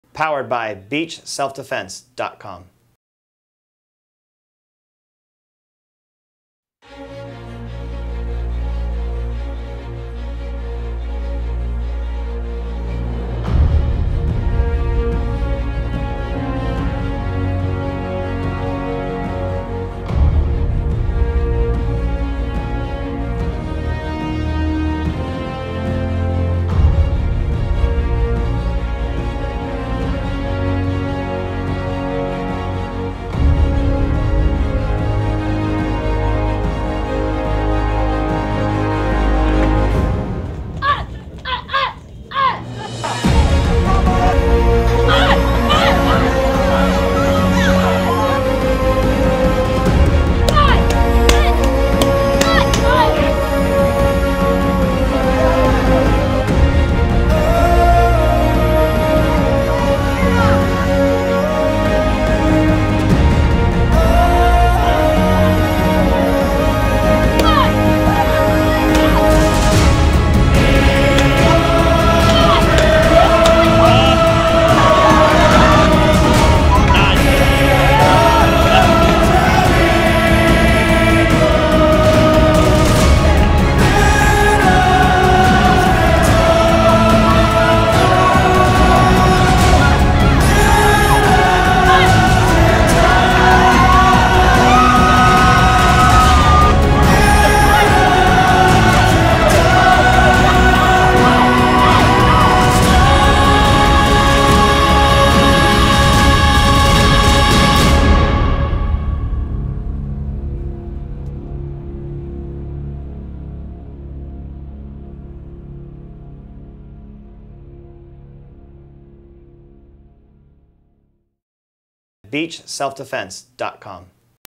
BC Krav Maga: The Warrior's Path
Footage from Junior Black Belt Testing.

krav maga...krav maga...krav maga.